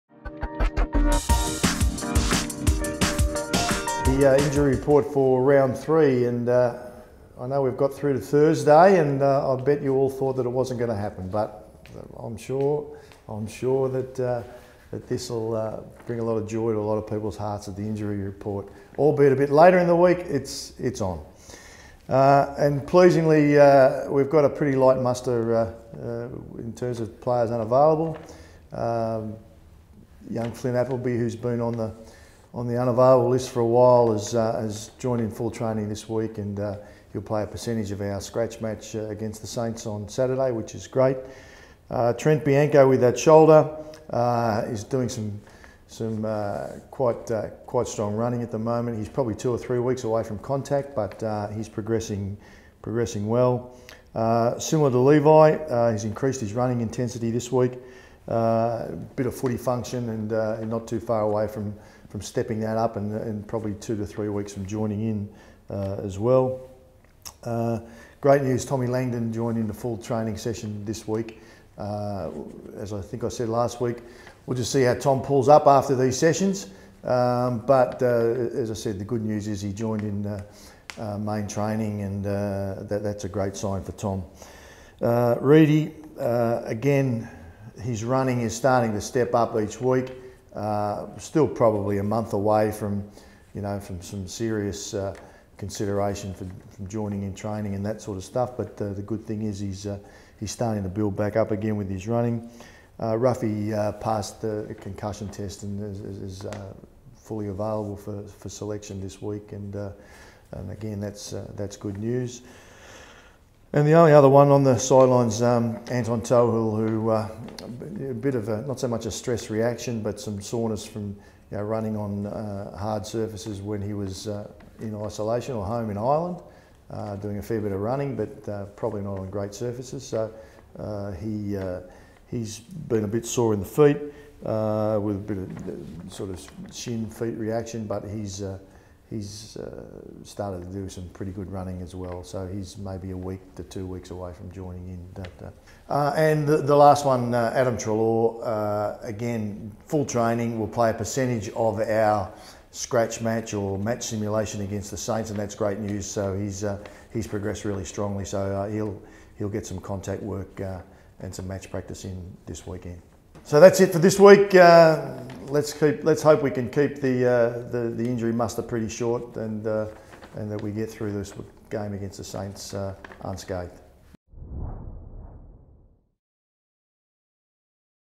Injury Report: Round 3
Hear from GM of Football Geoff Walsh as he discusses the teams health ahead of  our clash with the Saints on Saturday.